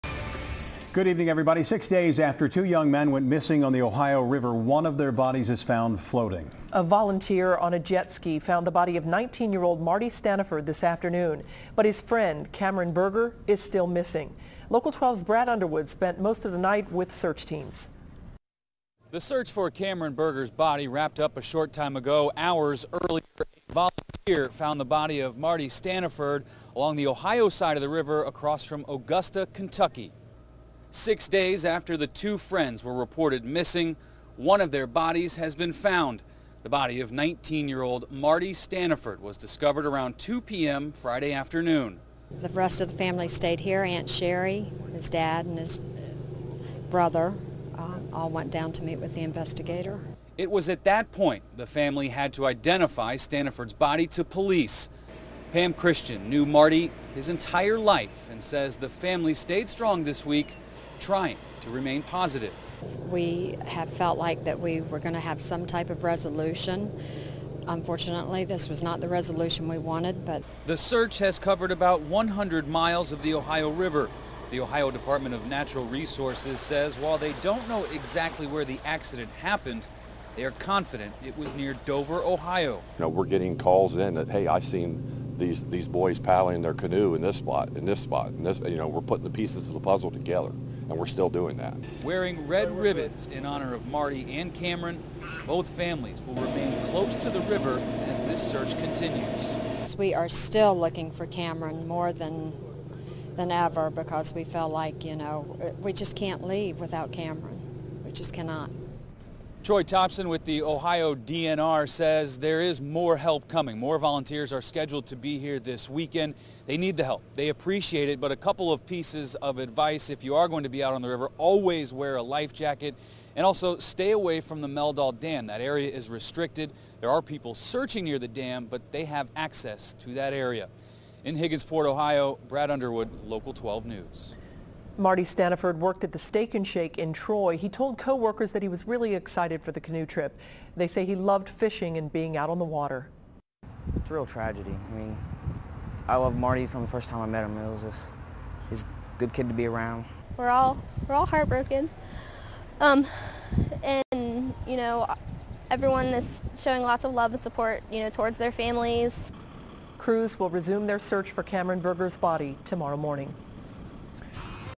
Crews still searching for second canoeist
BROWN COUNTY, Ohio (Brad Underwood) -- Six days after two young men went missing on the Ohio one of their bodies is found floating in the river.

A volunteer on a jet ski found the body of 19-year-old Marty Stanaford Friday afternoon around 2p.m.  But his friend, Cameron Buerger, is still missing.  The search for Cameron Buerger's body concluded for the night around 10:30p.m.  Six days after the two  friends were reported missing, one of them is still not found.

The family had to identify Stanaford's body to police.  Pam Christian knew Marty his entire life and says the family stayed strong this week, trying to remain positive.

She said, "We have felt like we were going to have some type of resolution.  Unfortunately this was not the resolution we wanted."

The search has covered about 100 miles of the Ohio River.  The Ohio Department of Natural Resources says while they don't know exactly where the accident happened, they are confident it was near Dover, Ohio.

Wearing red ribbons in honor of Marty and Cameron, both families will remain close to the river as the search continues.

Follow Brad Underwood on Twitter @BUnderwoodWKRC and LIKE him on Facebook